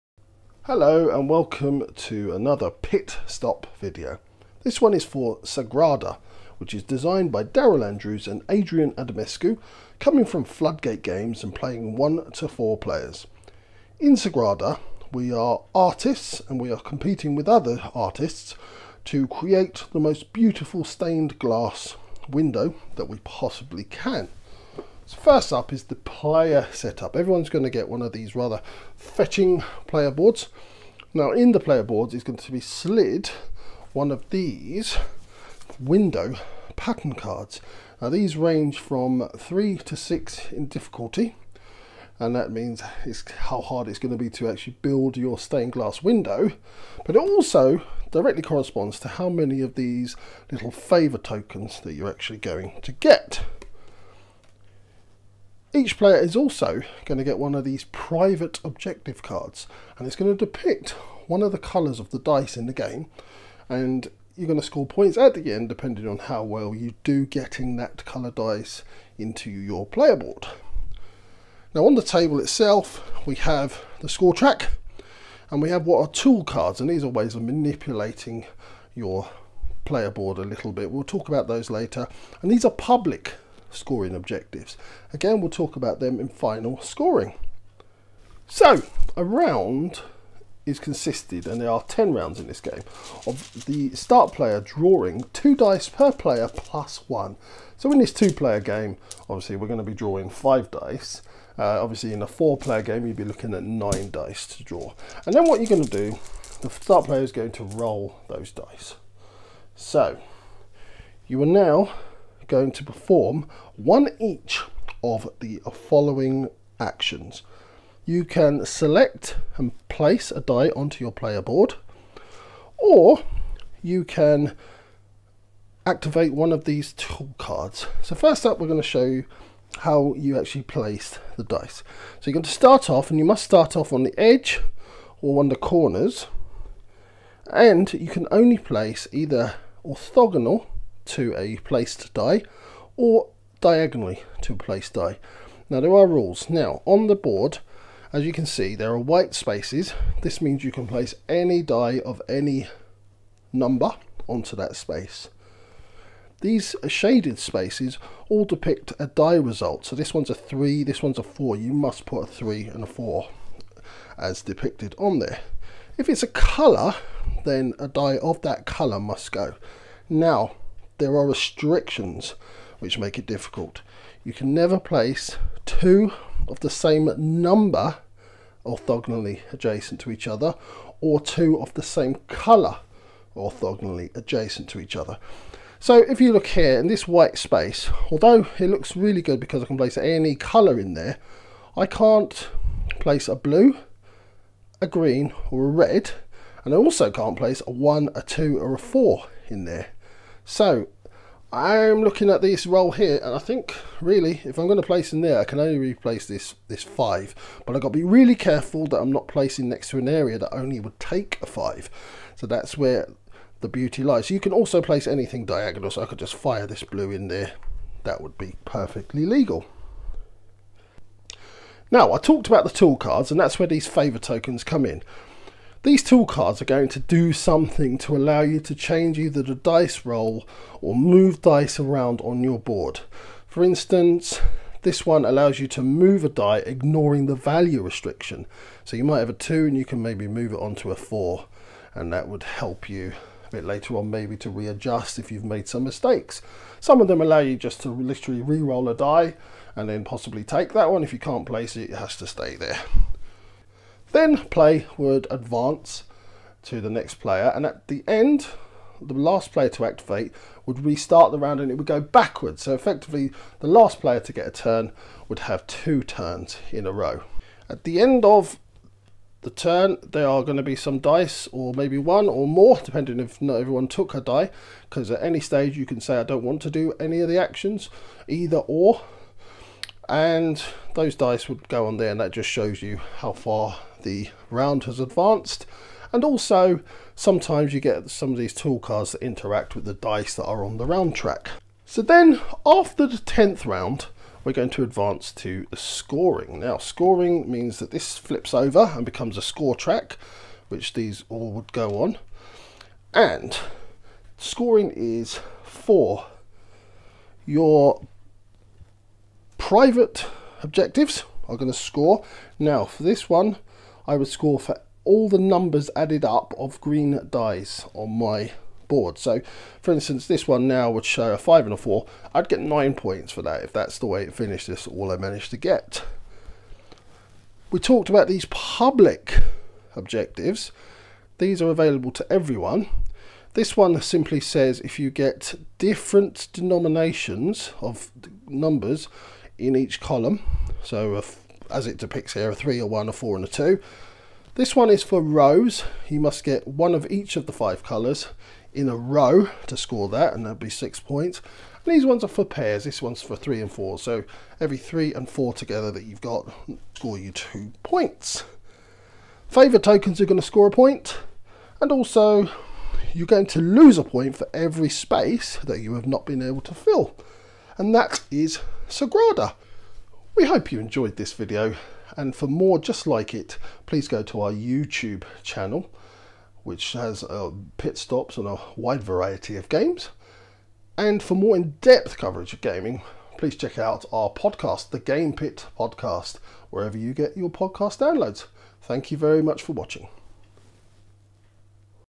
Pit Stop: Sagrada
Sean gives an overview on Sagrada from Floodgate Games and designed by Daryl Andrews and Adrian Adamescu.

For more in depth coverage on board and card gaming check out The Game Pit Podcast: